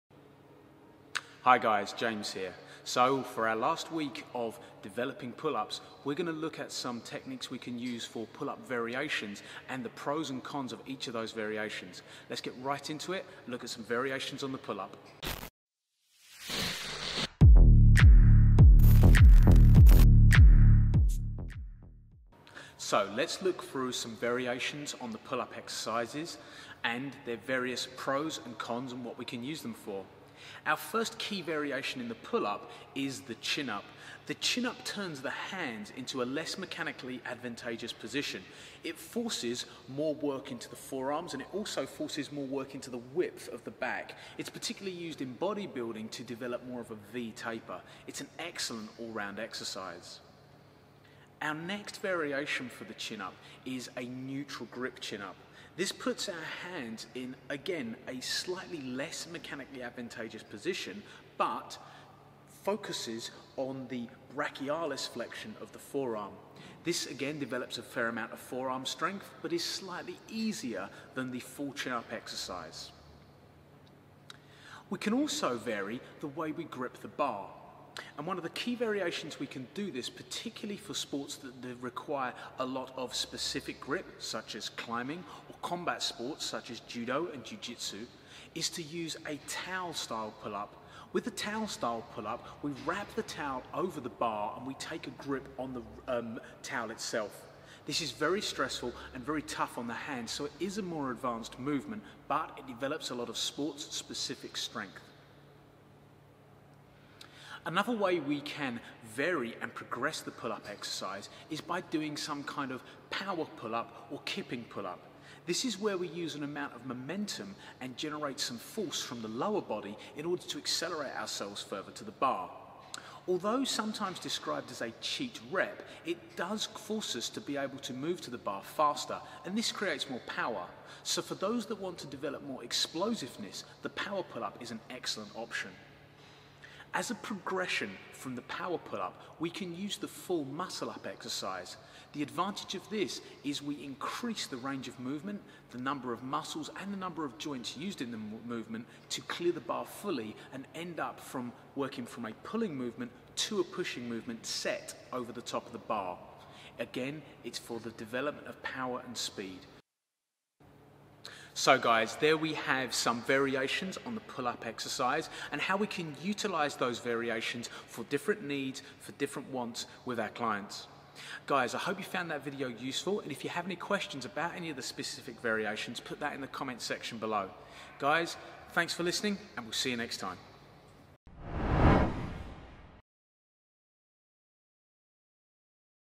How to Master the Pull Up: Episode Four
This is the fourth and final episode in our series on 'how to master the pull up'. In this weeks episode James look at different variations of the pull up exercises, progressing to a chin up and muscle up. Discover the correct way to execute these exercises as well as more challenging alternatives!